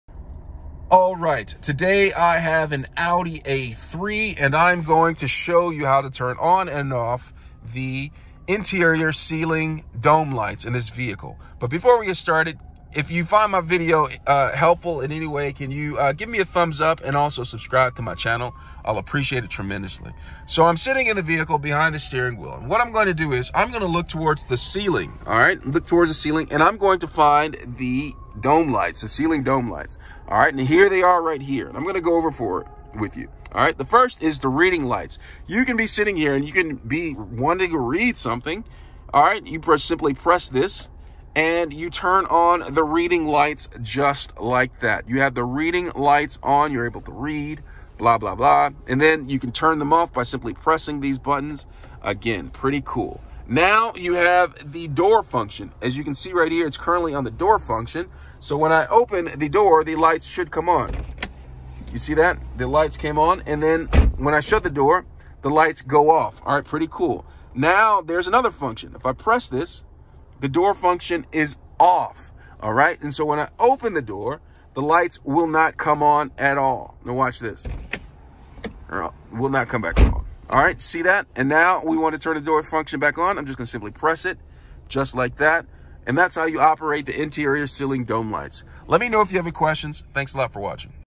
Audi A3 – how to turn on/off interior ceiling dome lights
Demonstrating how to turn on and off the interior ceiling dome lights of a Audi A3. .